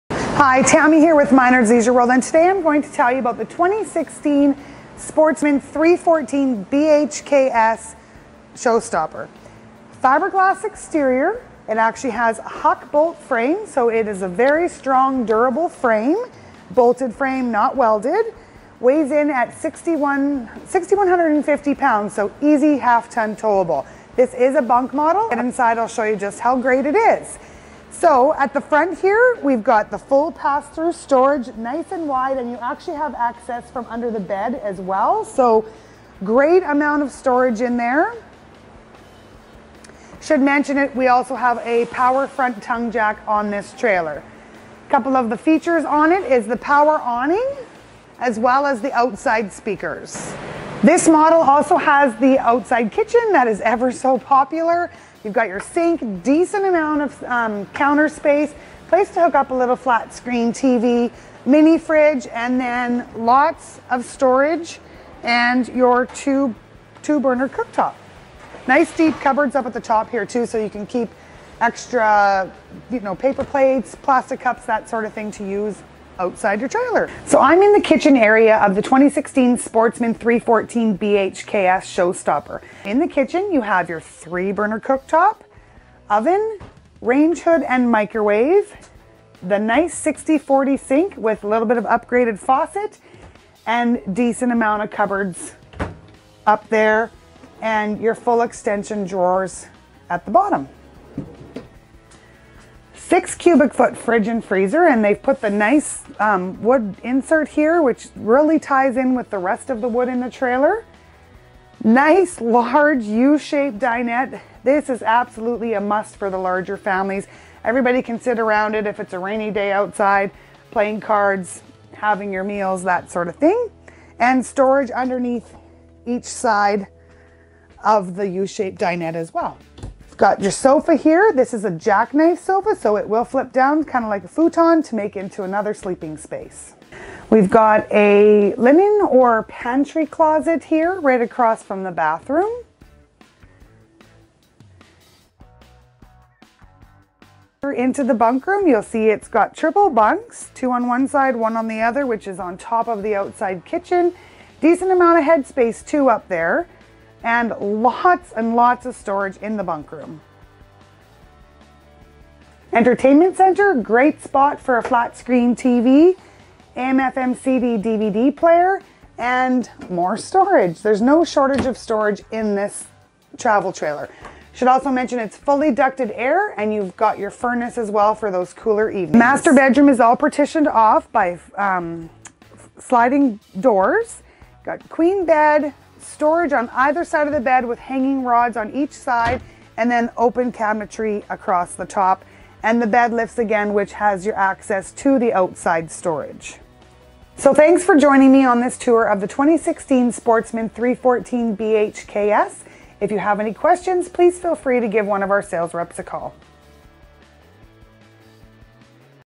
2016 Sportsmen Show Stopper 314 BHSS by KZ
Terrific value, the Sportmen product, with great upgrades at a fabulous price! Fiberglass exterior, power tongue jack,outside kitchen and outside speakers. The interior has triple bunks,large U dinette and jackknife sofa along with a Queen bed in the master!